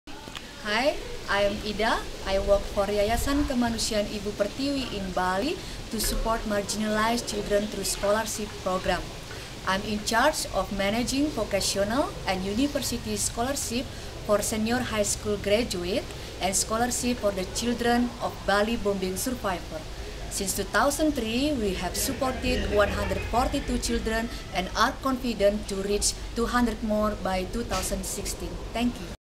Ida introduces YKIP education and scholarship programs for Bali's disadvantaged
In 30 seconds or less, Ida from YKIP, in Bali, Indonesia, introducing education and vocational scholarship programs for Bali's disadvantaged, including families affected by Bali's terror bomb blasts.